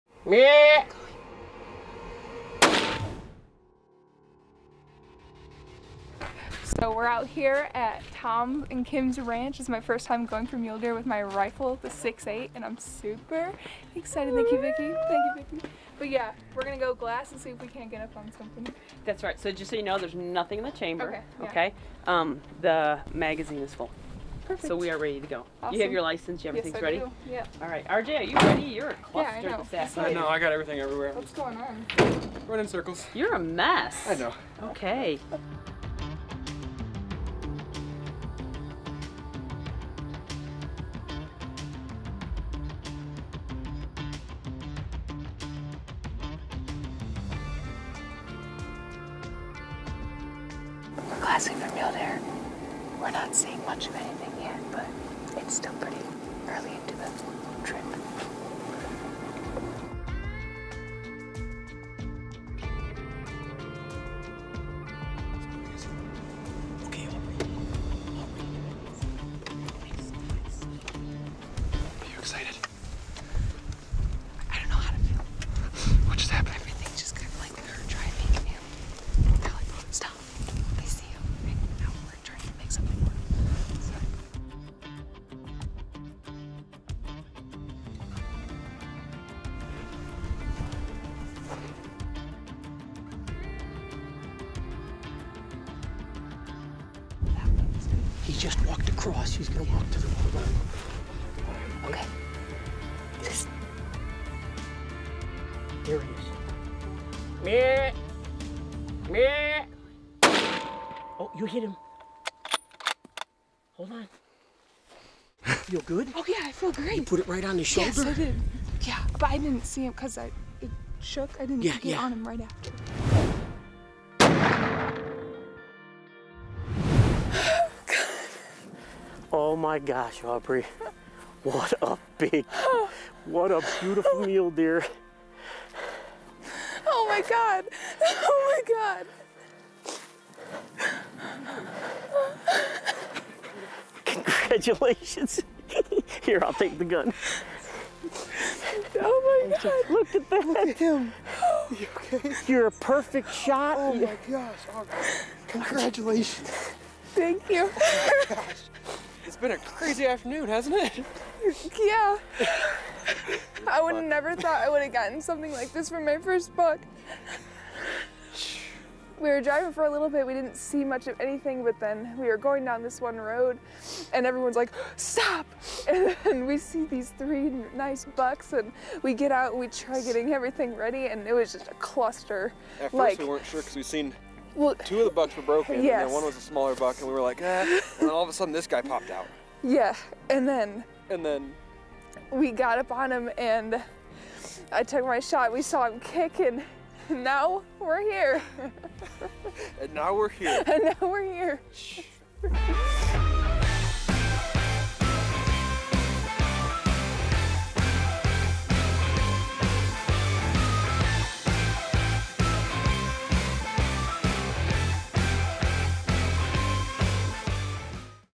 Aubrey's First Mule Deer Buck! - The Choice Hunts
Join us as we follow Aubrey's first successful pursuit of a Mule Deer Buck!